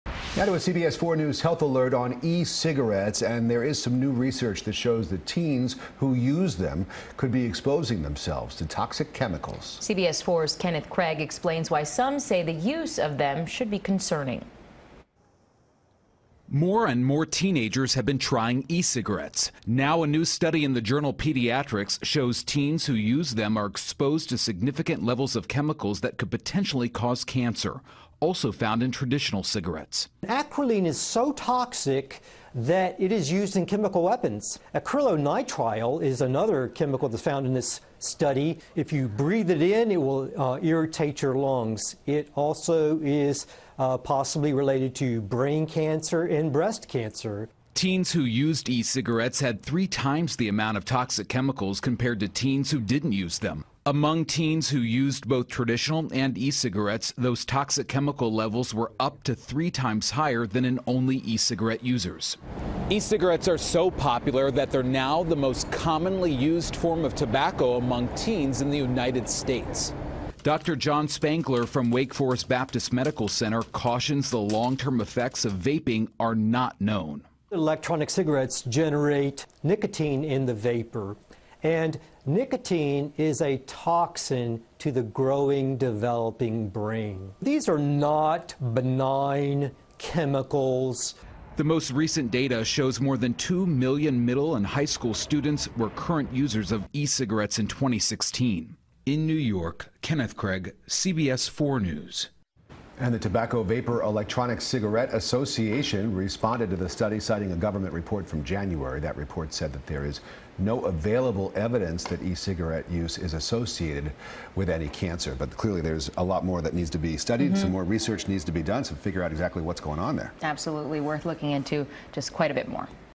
E-Cigarettes Have Chemicals That Are Especially Dangerous For Teens
CBS Miami's Kenneth Craig reports on how e-cigarettes can be extremely dangerous, especially to teenagers.